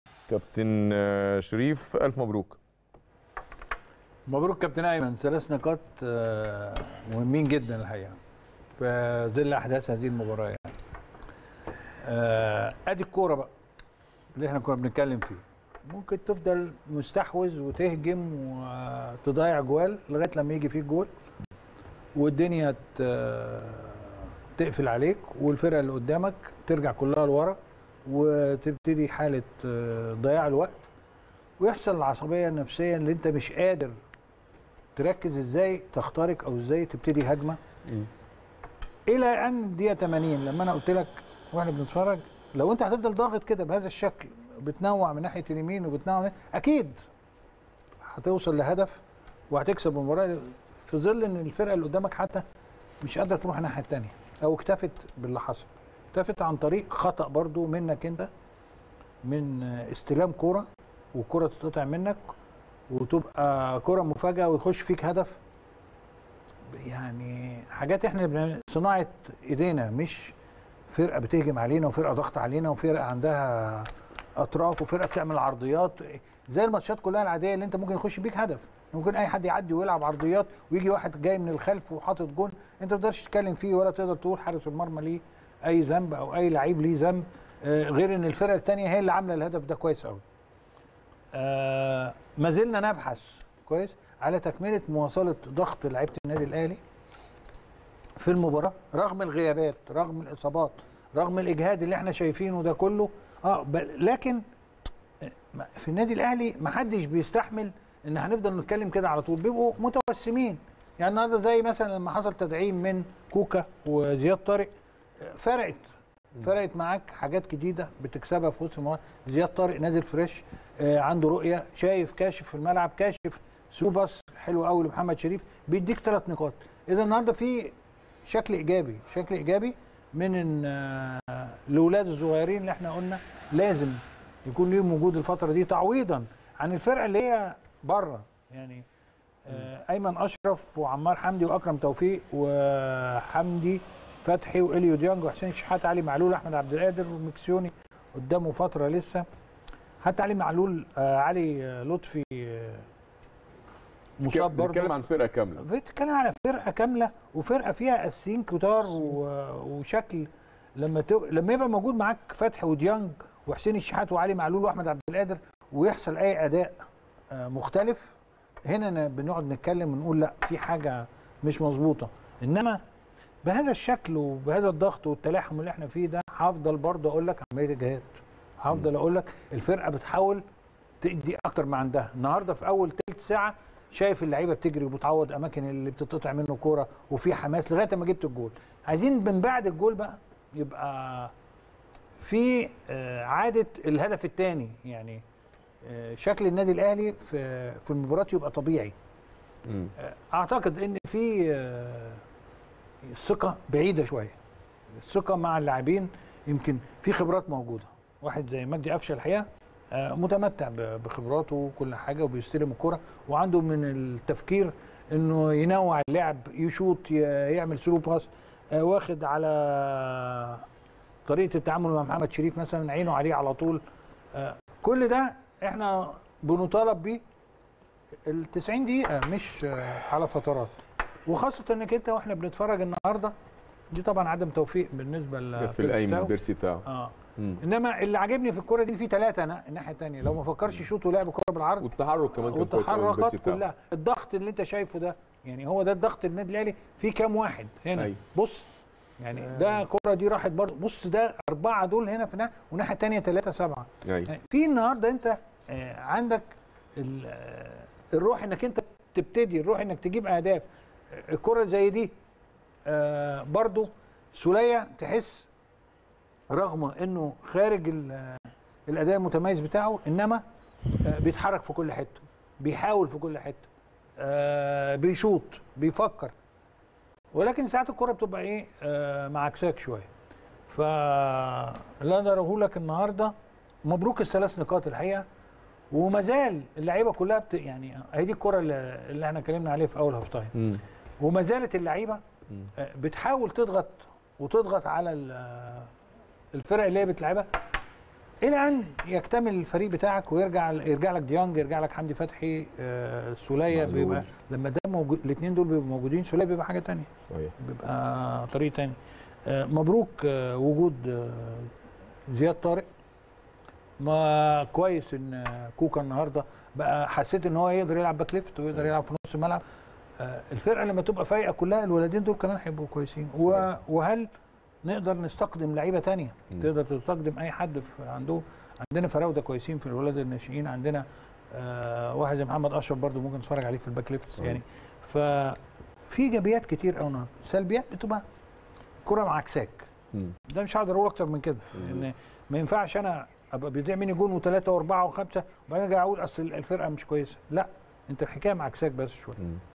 شريف عبد المنعم يعدد ايجابيات الفوز علي غزل المحلة في ظل الغيابات التي يعاني منها الفريق
شريف عبد المنعم يعدد ايجابيات الفوز علي غزل المحلة في ظل الغيابات التي يعاني منها الفريق | الاستوديو التحليلي